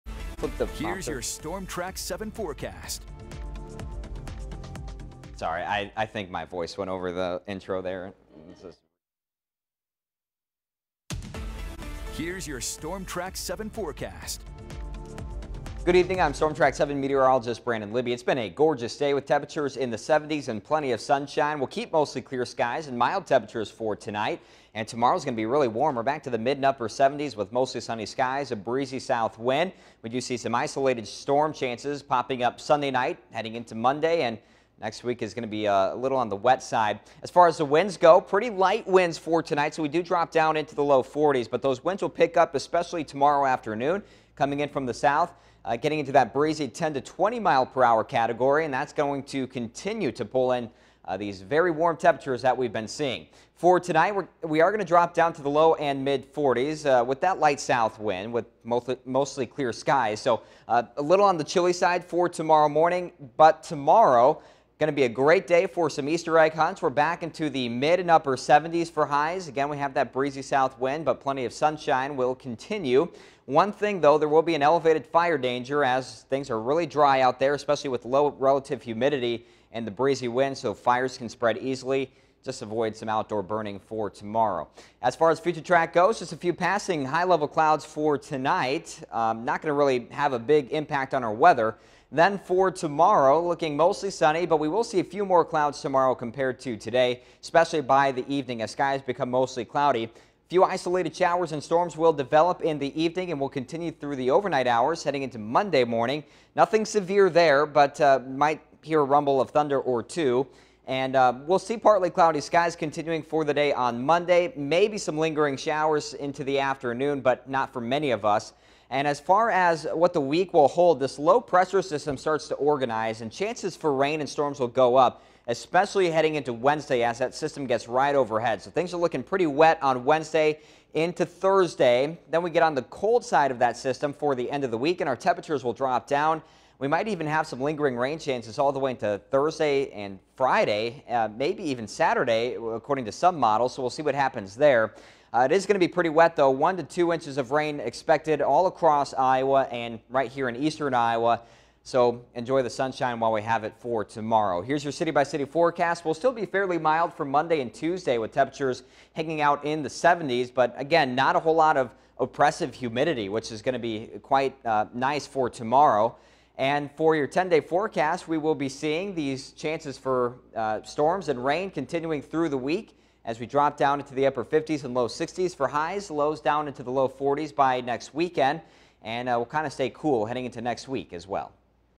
Evening Weather Sat. 4/3/2021
Nice and warm for Easter Sunday.